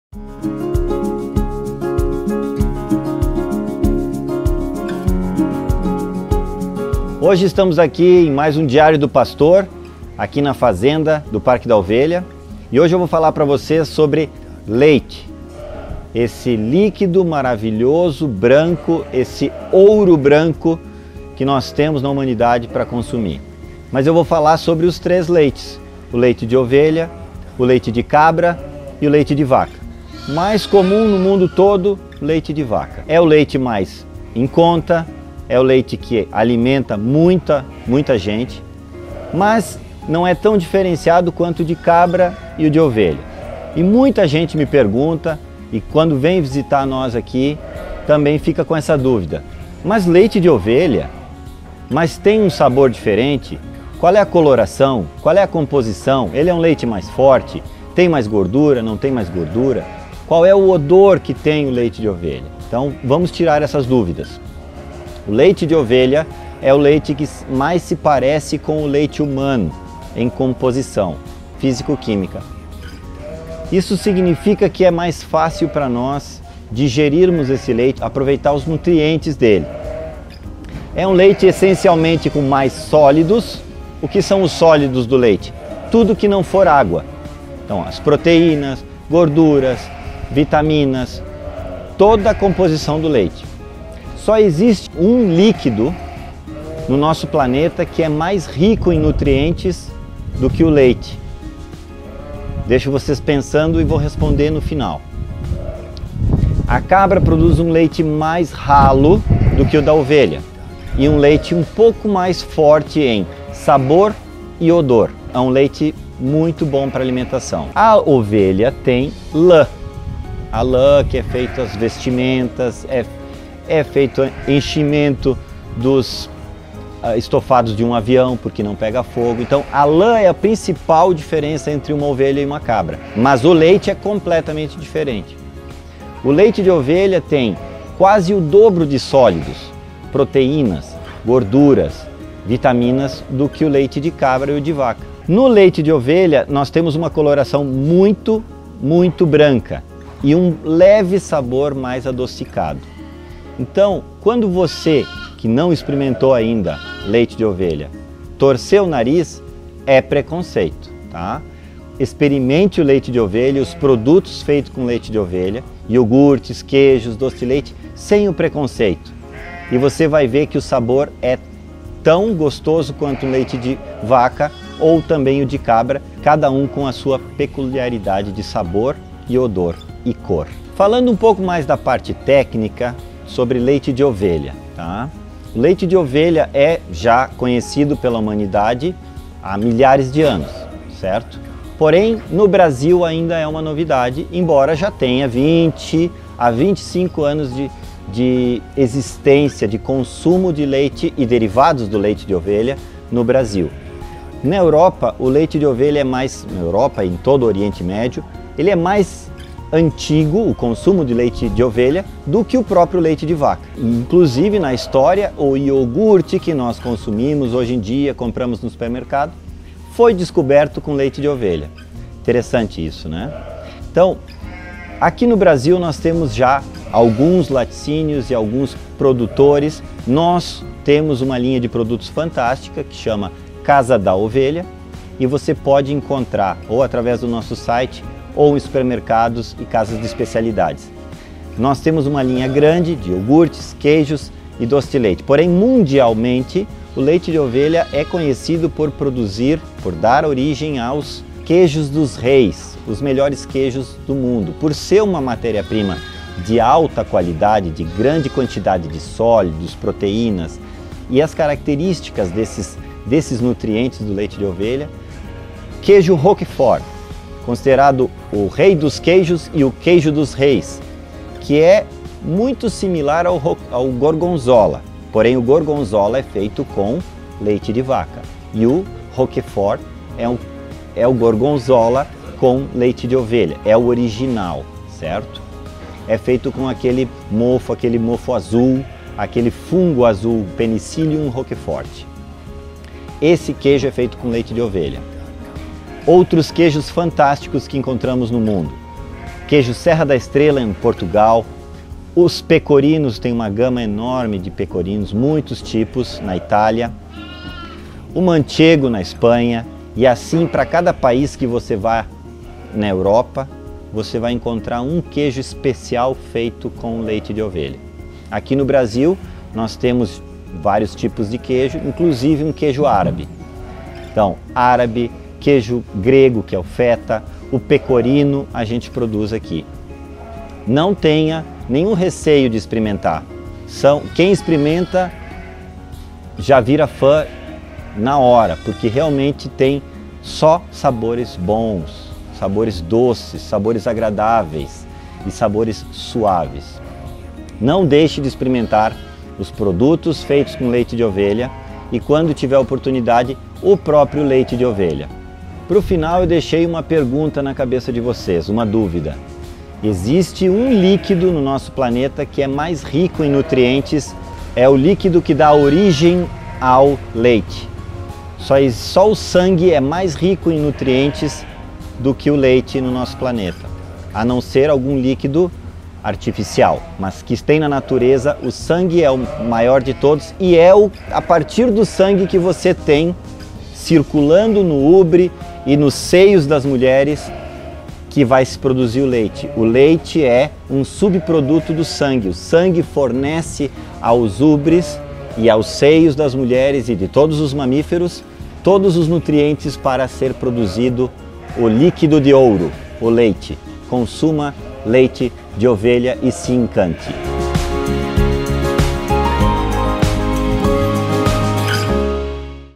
Leite de Ovelha | Episódio 15 | Diário do Pastor
Você conhece o Leite de Ovelha?

Neste novo episódio do Diário do Pastor, trazemos para você informações sobre a nossa matéria-prima, o leite de ovelha!

Ele é muito nutritivo e delicioso, apresentando uma coloração branca intensa e homogênea, com um sabor levemente adocicado e suave, além de uma cremosidade única.

Ele é repleto de inúmeros benefícios, confira:
• Proteínas: Alto teor de proteínas no Leite de Ovelha, em relação a outros tipos de leite. Apresentando também um alto valor biológico.
• Gorduras: Possui quantidades maiores em relação ao leite de vaca e cabra, porém elas são gorduras menores, que proporcionam maior digestibilidade.
• Minerais: Contém 50% mais de cálcio, comparado ao leite de vaca.
• Vitaminas: Contém vitamina A, B1, B2 e B12. Além de Vitamina C, com teor de 150% a mais, e a Biotina com teor de 160% a mais, em relação ao leite de vaca.